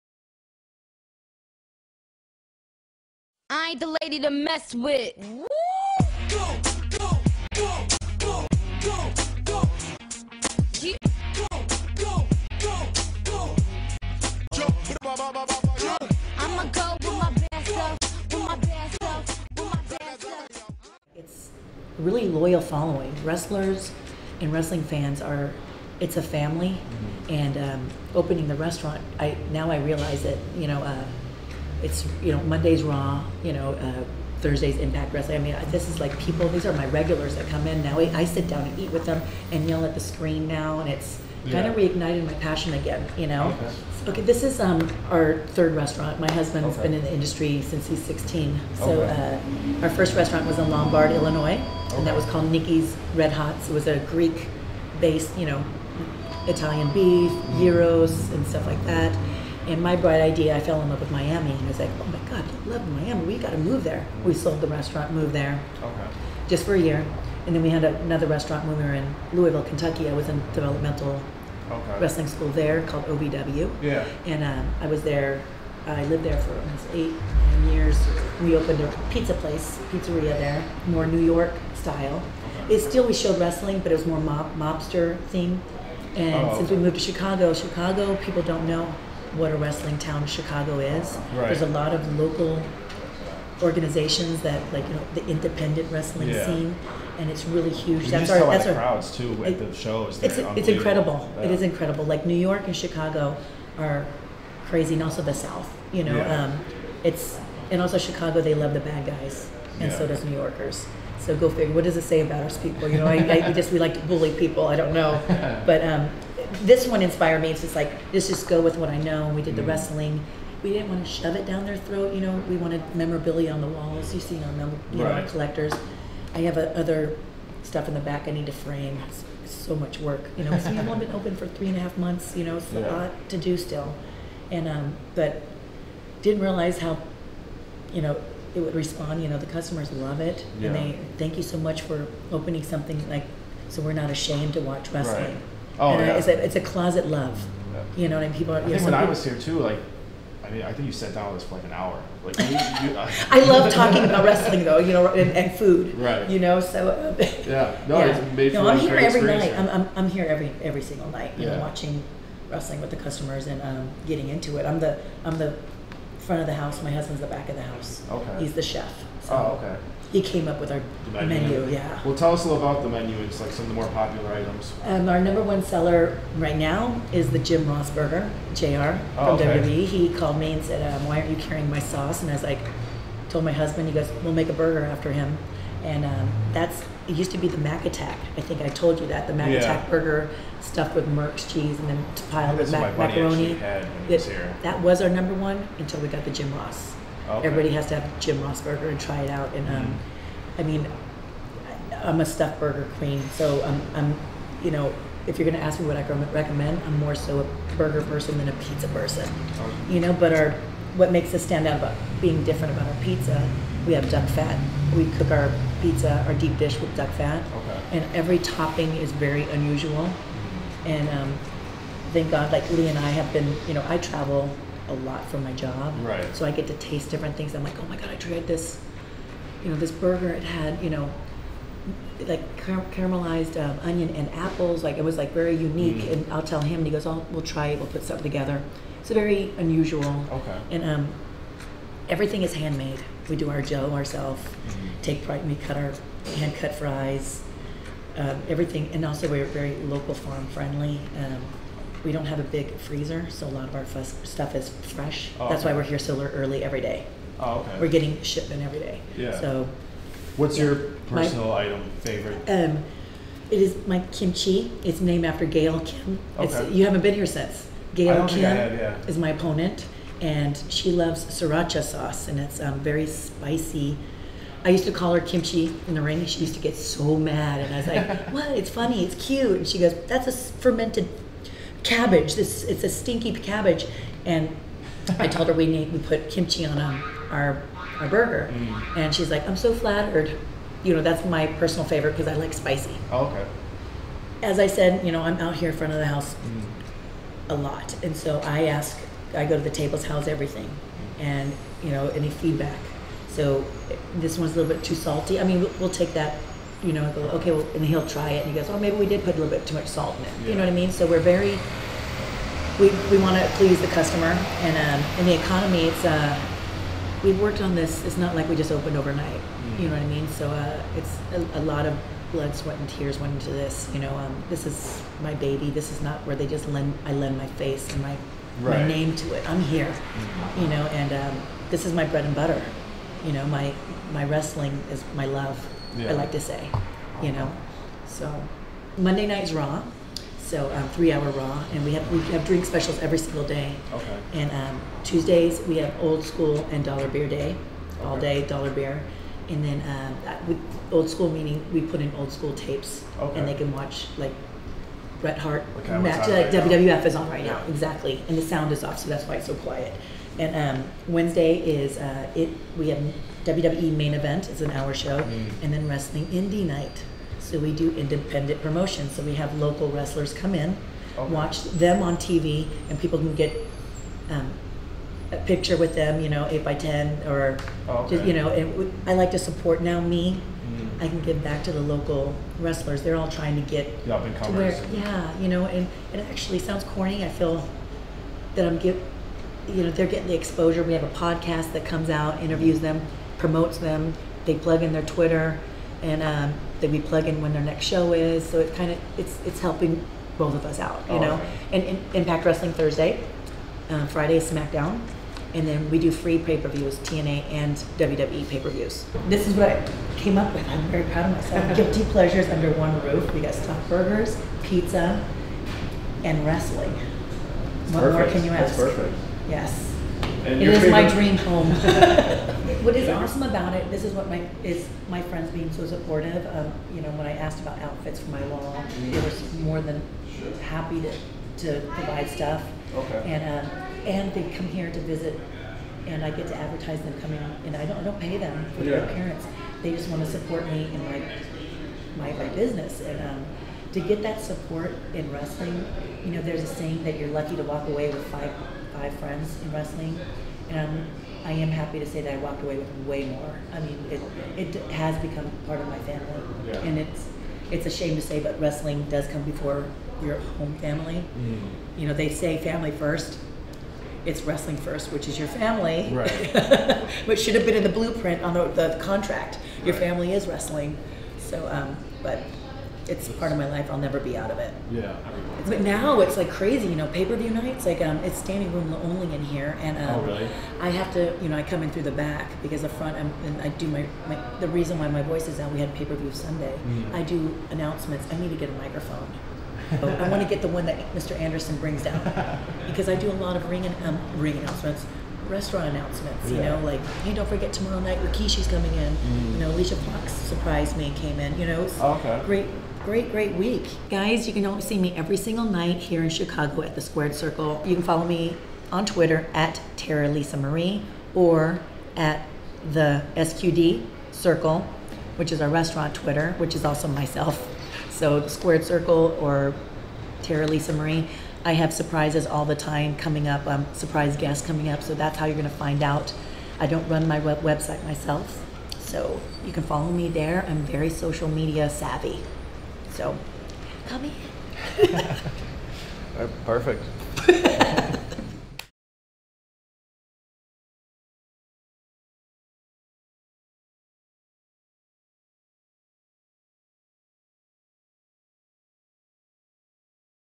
The Squared Circle Restaurant - WWE's Lisa Marie Varon
SportsMockery.com EXCLUSIVE:

Lisa Marie Varon is professional wrestler who is also making a name for herself in the restaurant business. The former two time WWE Women's Champion and five time TNA Women's Knockout Champion, owns and operates The Squared Circle with her husband Lee Varon.

The Squared Circle is located at 2418 N. Ashland in Chicago, and is a one of a kind restaurant. They offer craft beers, a full bar, brick oven pizza, and gourmet stuffed burgers. In addition, The Squared Circle offers wrestling every night from WWE's Raw, Smackdown, TNA's Impact Wrestling, and all of the year's pay per views. On nights where live wrestling is not on television, they have old school wrestling on the televisions.